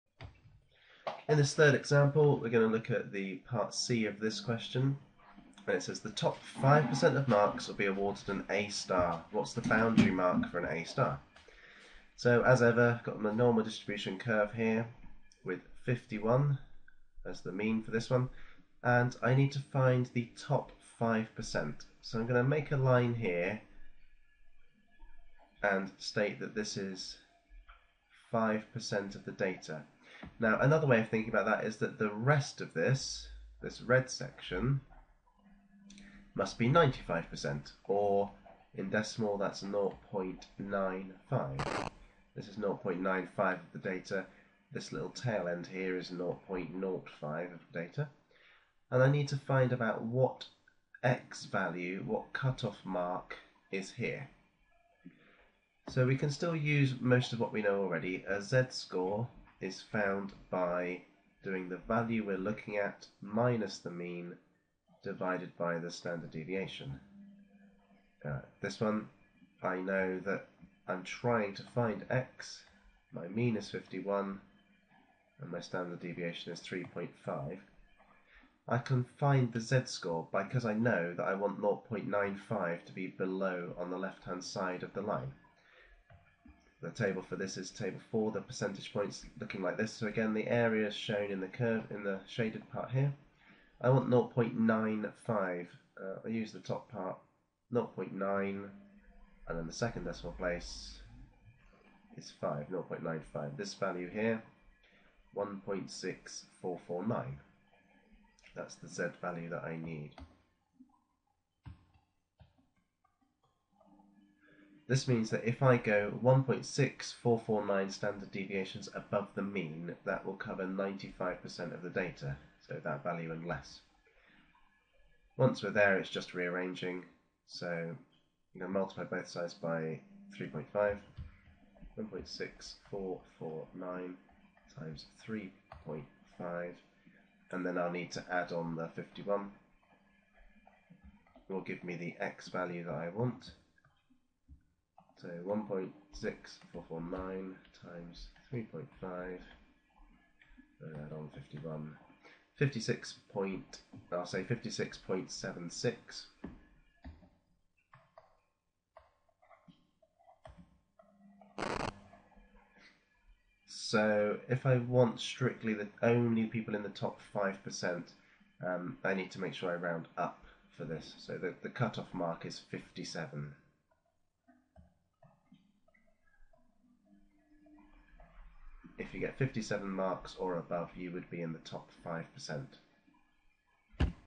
Normal Distribution Example 3 (Reverse z value problem)
In this video, I go through an example of calculating a data value given the percentage of the population we want to cut off. We use the Normal Distribution percentage points table to find the required z value and then work backwards to find the value.